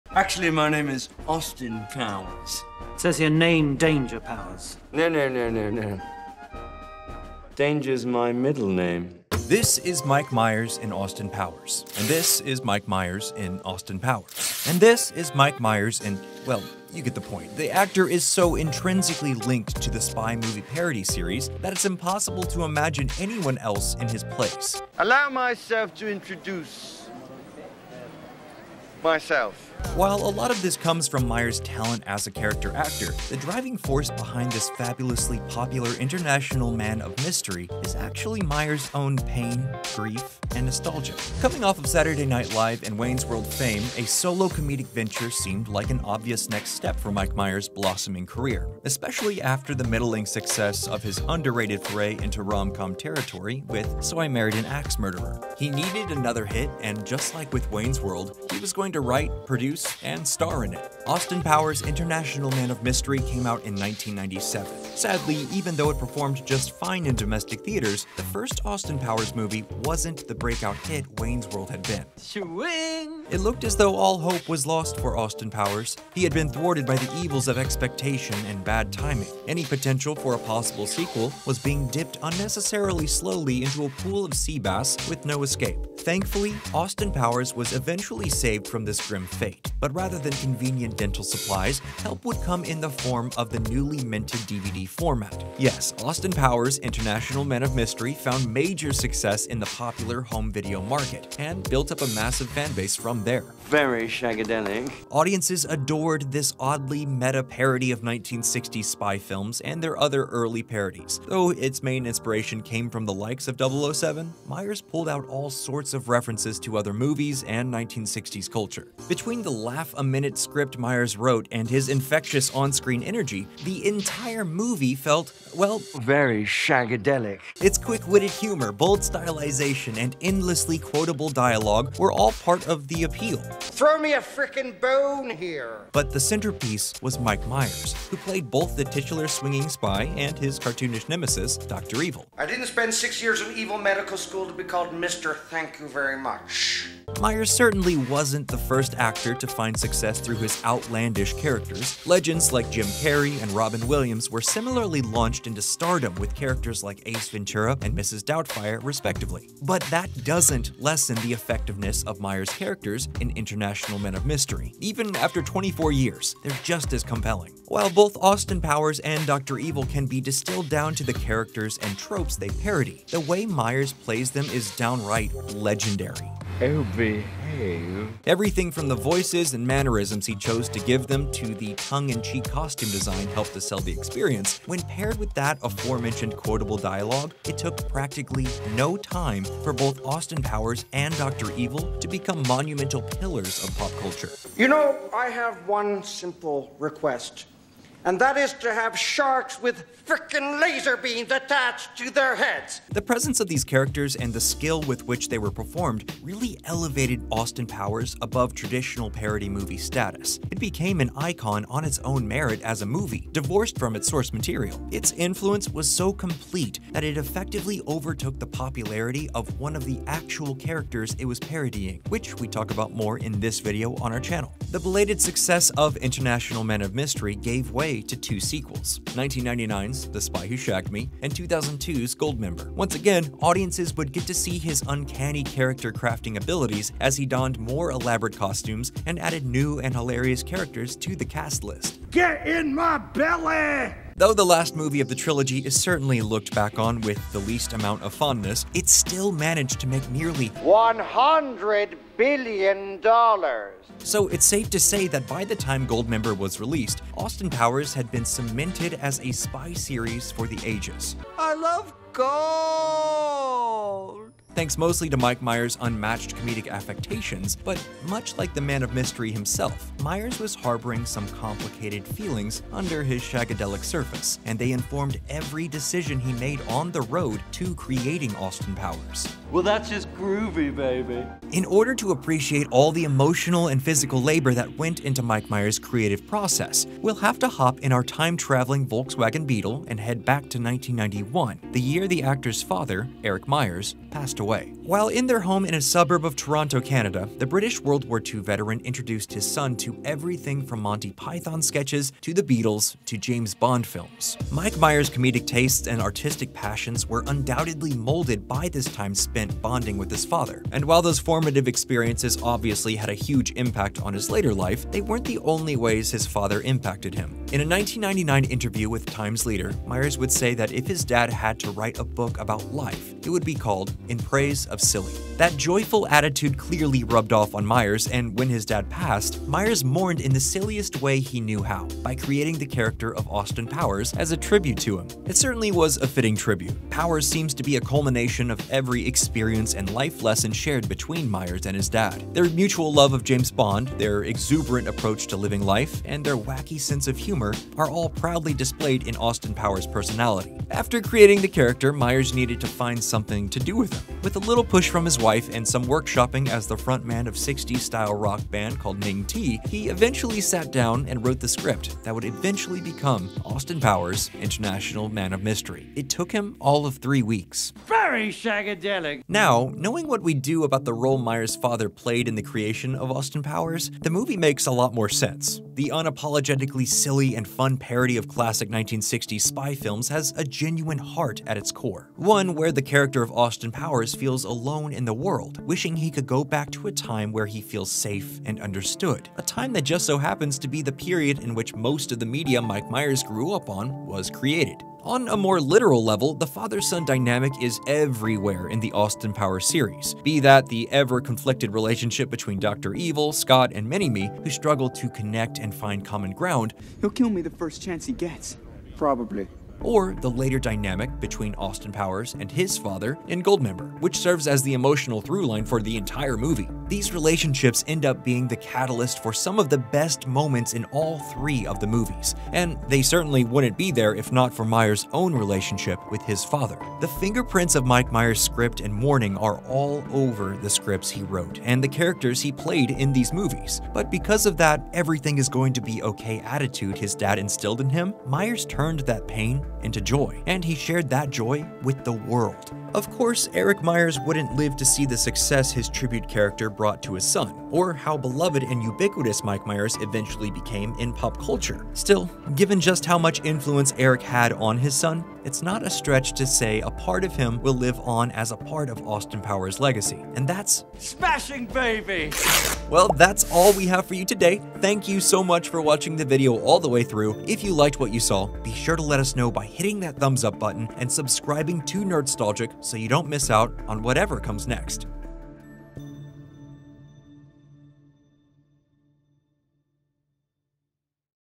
Why Mike Myers Is One Of The Only Actors You Can't Replace
Ever since International Man of Mystery premiered, Austin Powers, Dr. Evil, and the many other characters played by Mike Myers are still household names.  Wish some going so far as to become more popular than the James Bond characters Mike Myers was parodying.  Only the legendary Mike Myers could have brought these characters to life, only his silly genius could have created a franchise that stands the test of time.